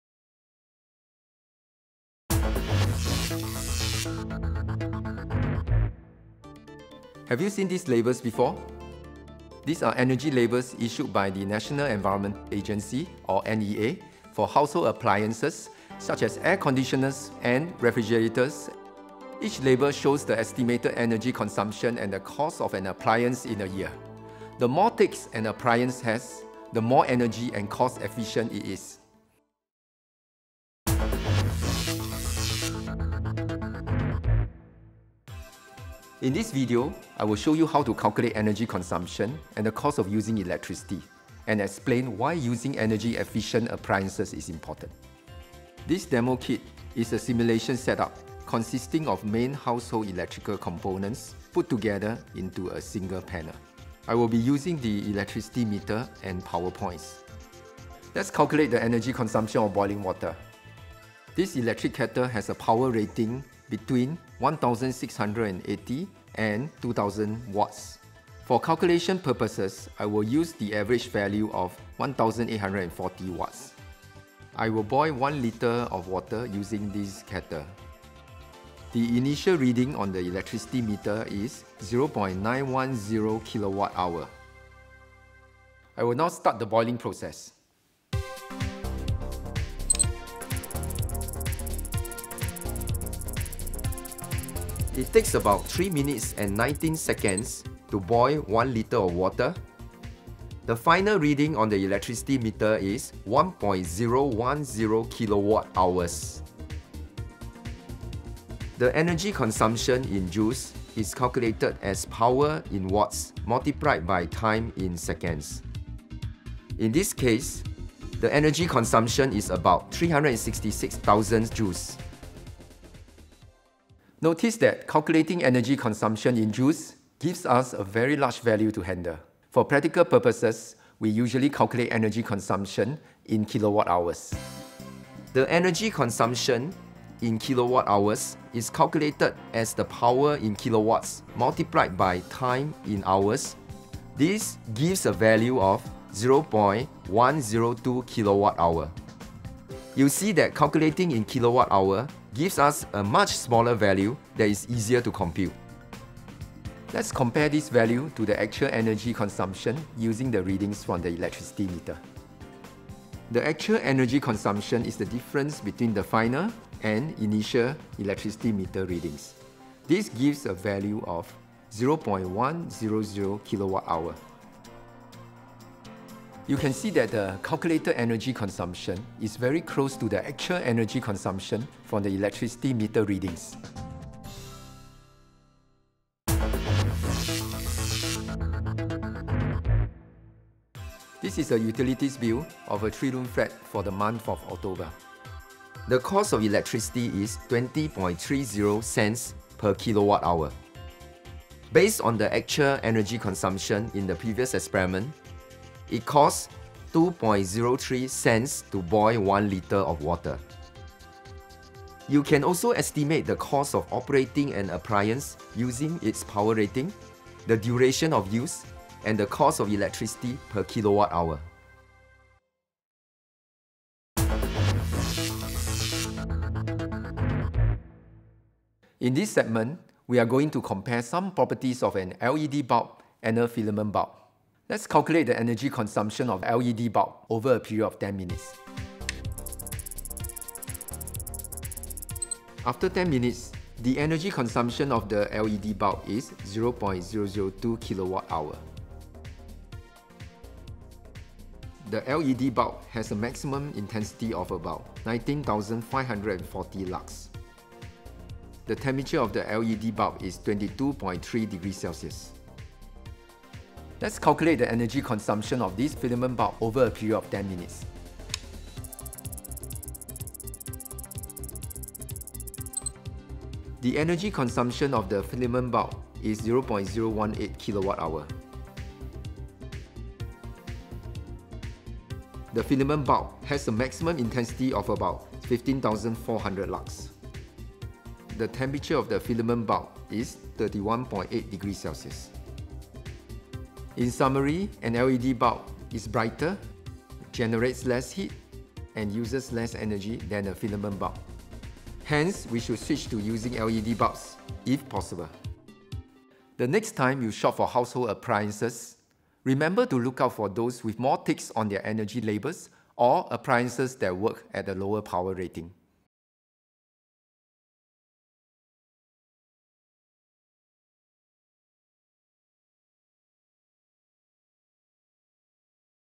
Household Energy Consumption and Cost of Electricity
This video shows how household energy consumption and cost of electricity are calculated.